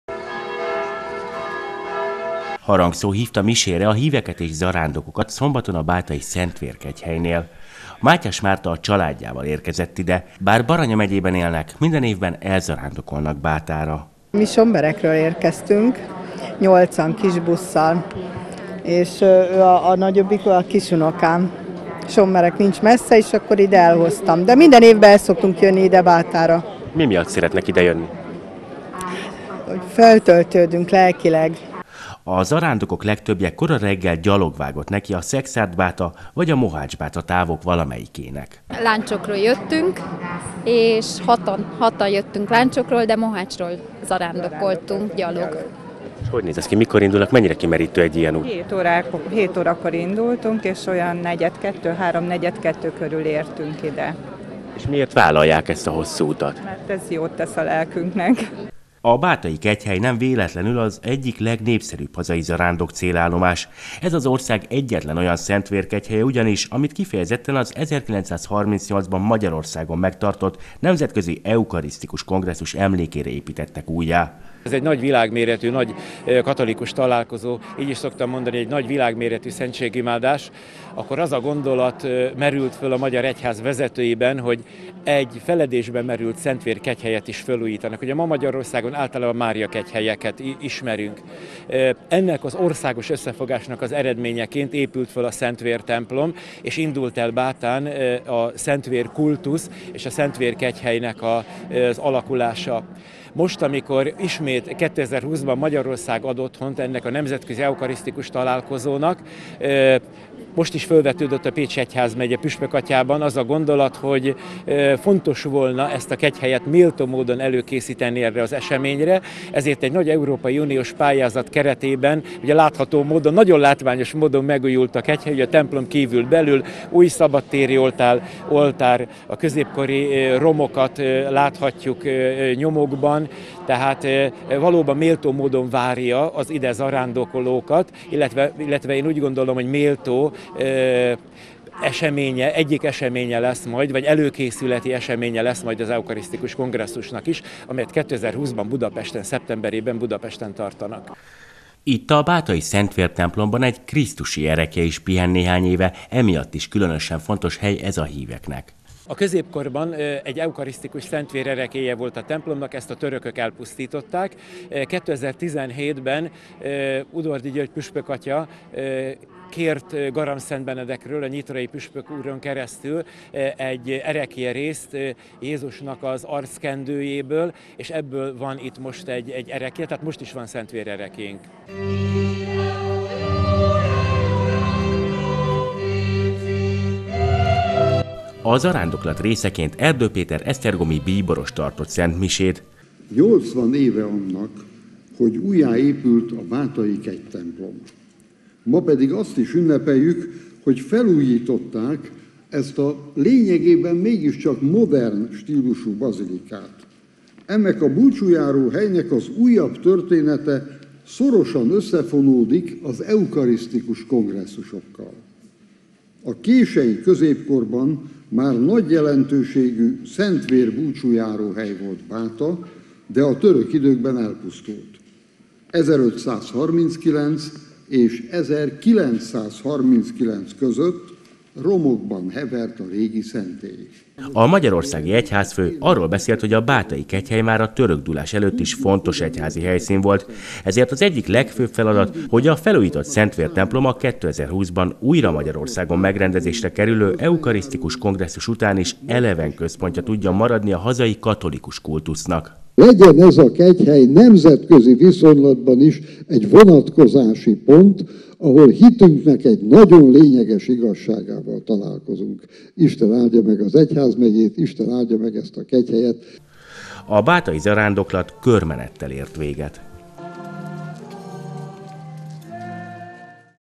Több mint kétezer zarándok vett részt a bátai Szent Vér templomnál megtartott szentmisén 20190916
Több mint kétezer zarándok vett részt a bátai Szent Vér templomnál megtartott szentmisén. A misét Erdő Péter bíboros celebrálta.